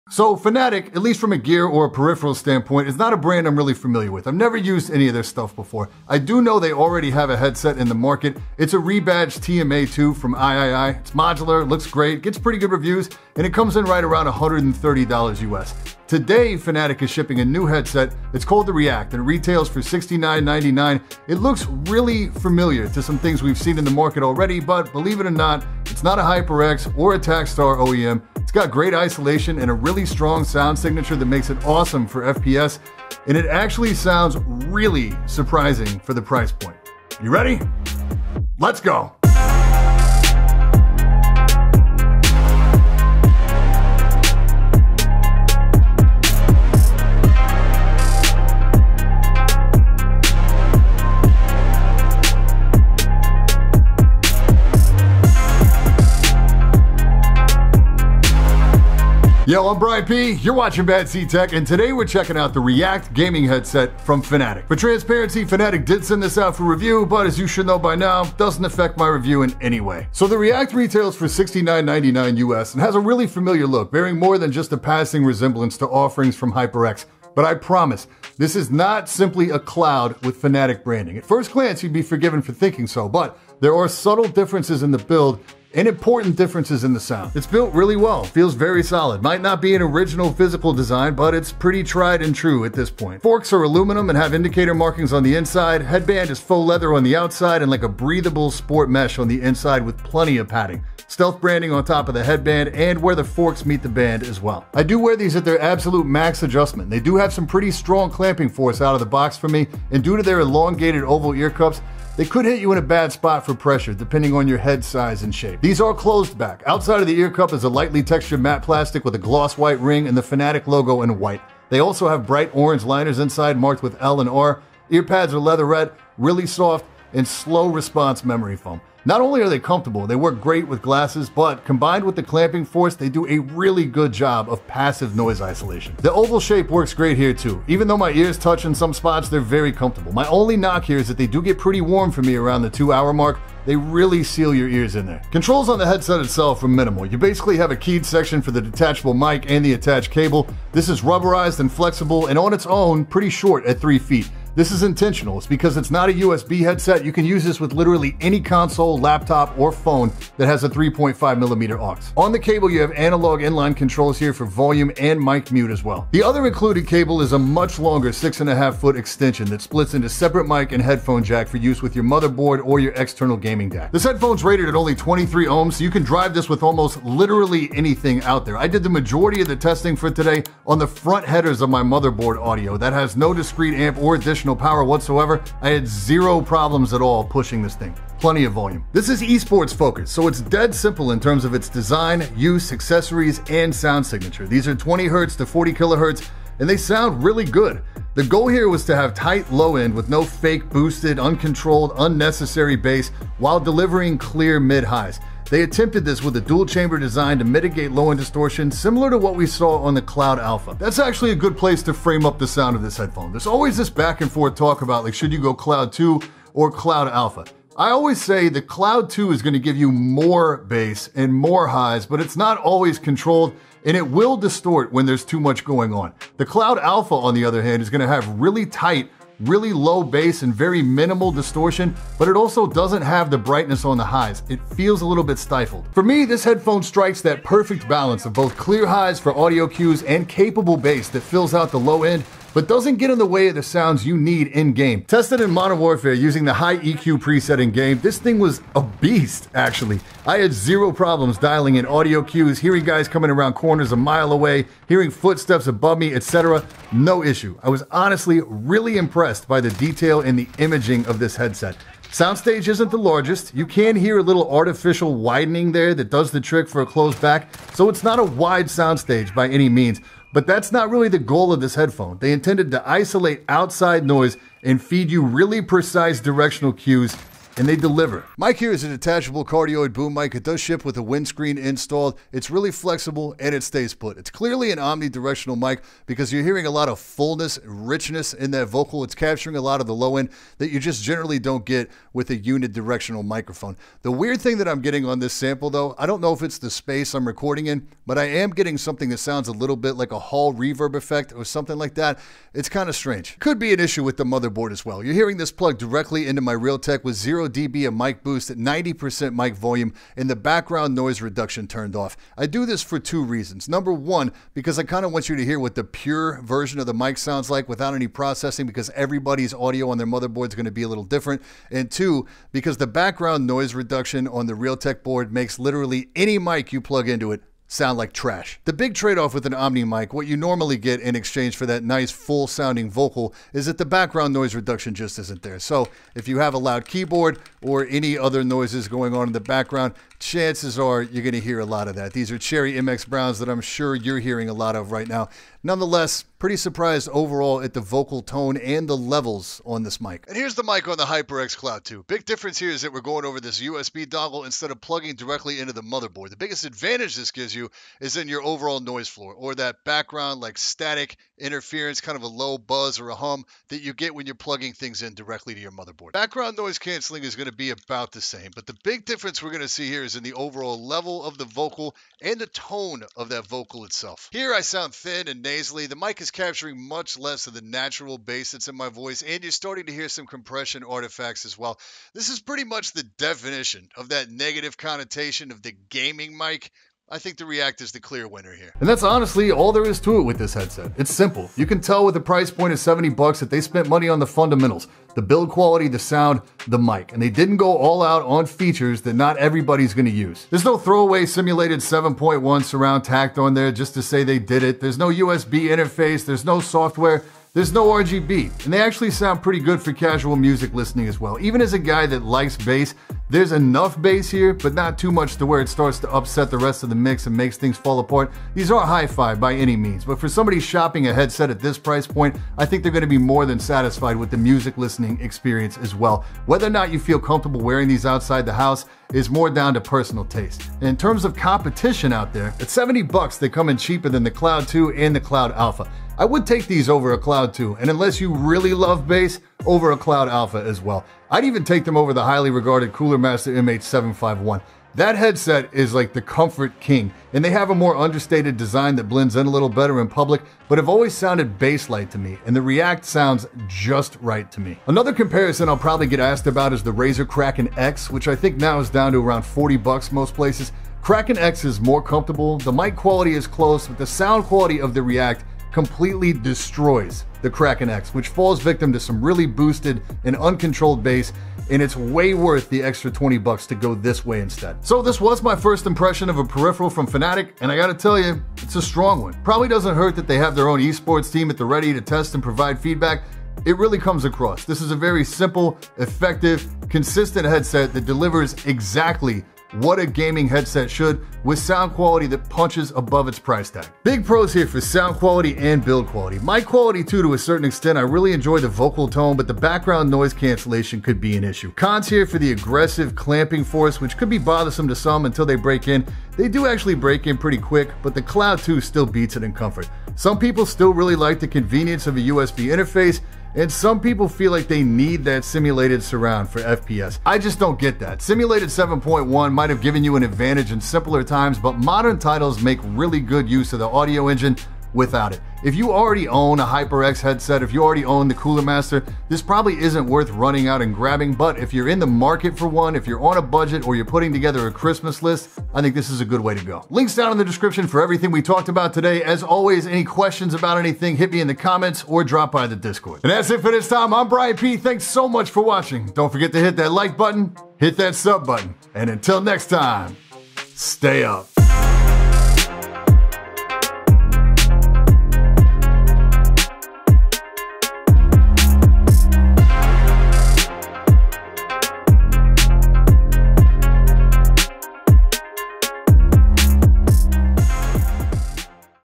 Fnatic REACT Gaming Headset Review: Budget Price BIG Performance
Check out me review of the Fnatic REACT Gaming Headset.  This is a budget offering with a mic and sound quality that is honestly worth way more than its price tag.

✅Not sold on this product? Check out Lustre's curated top list of similar products from other trusted reviewers